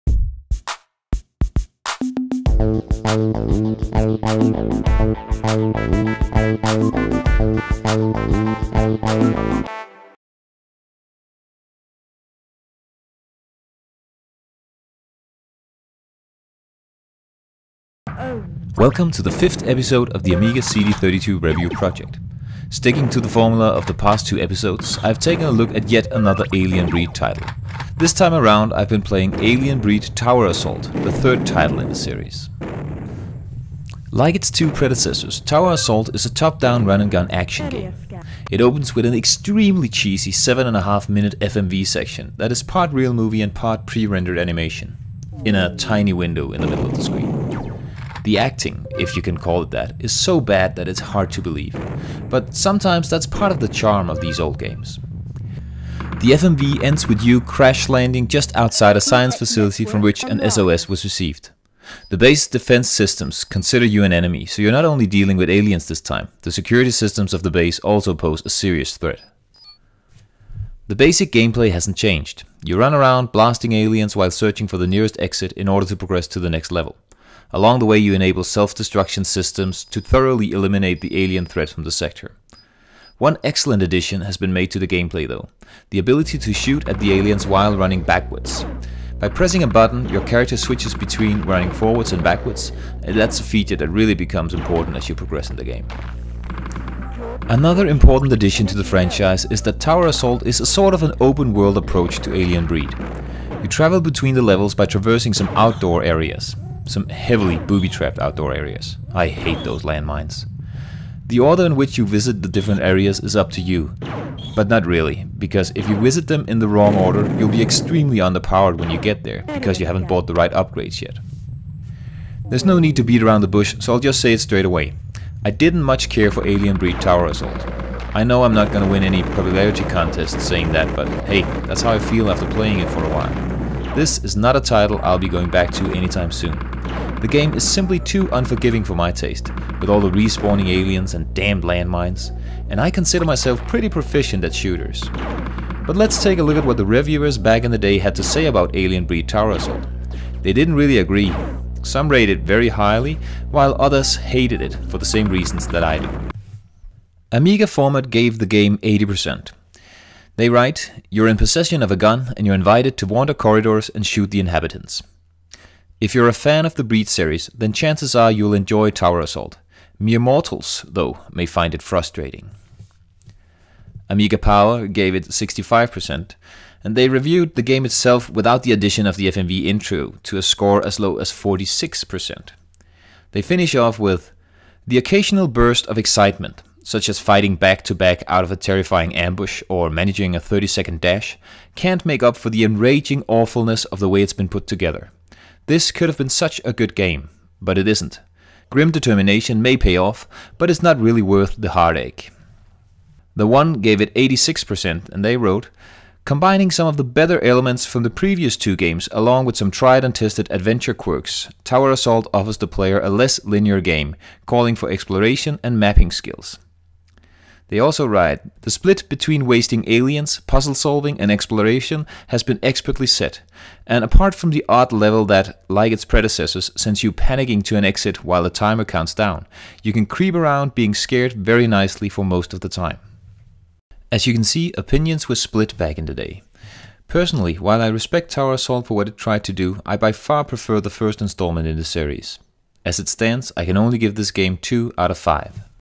Alien Breed Tower Assault
In this, the fifth, episode of the Amiga CD32 Review Project we take a look at Alien Breed Tower Assault.